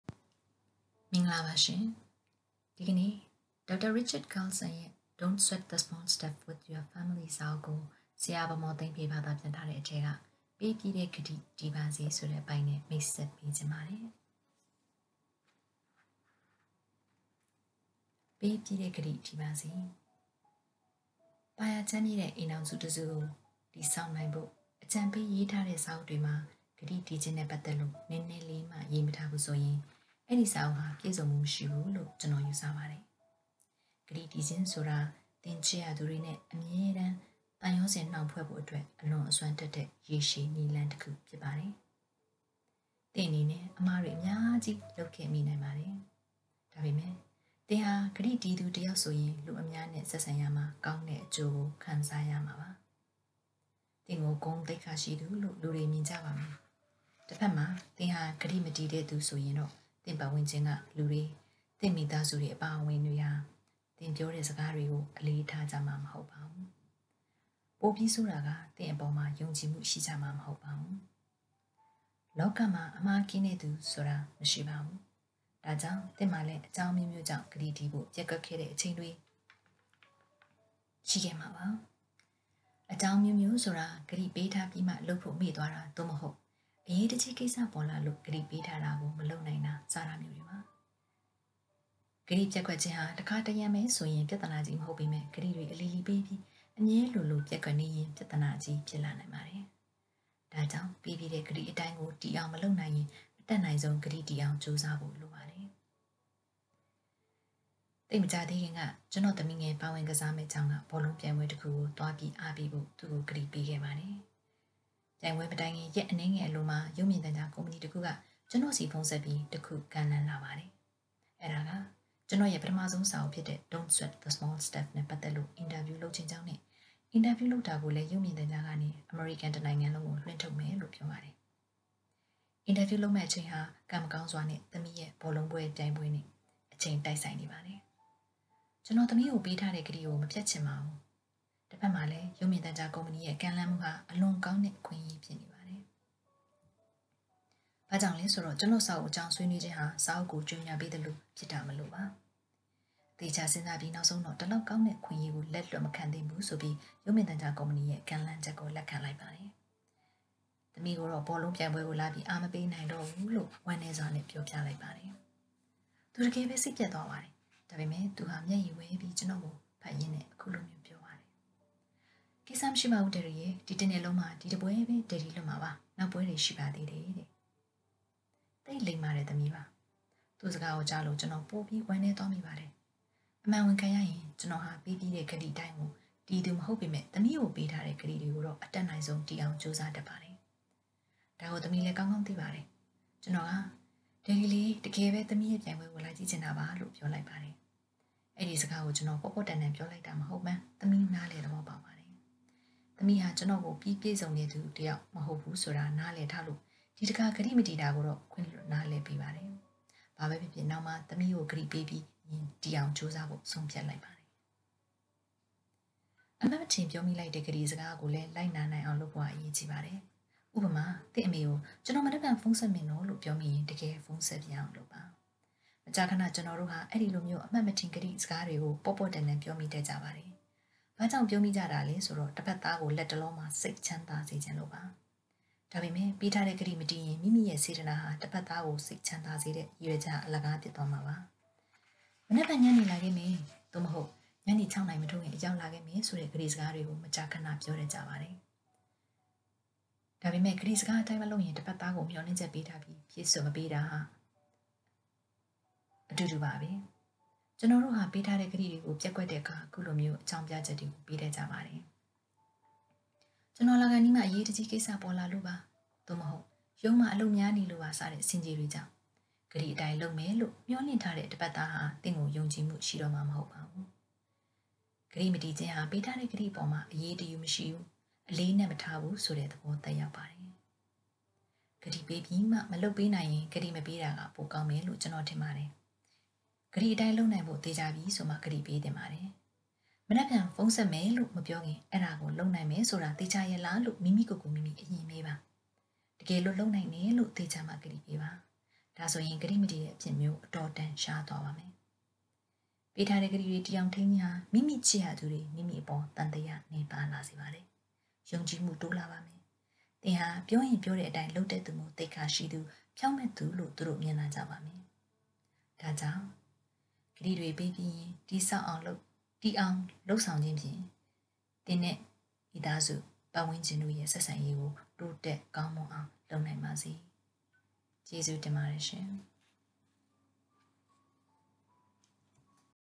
Don’t Sweat the Small Stuff with your family: To honor a promise
Dr. Richard Carlson's books offer simple and insightful ways to reduce stress and find peace in our daily life. In particular, Don't Sweat the Small Stuff with Your Family is filled with helpful tips for managing daily annoyances. Now, I will read a few key passages from this book in Burmese!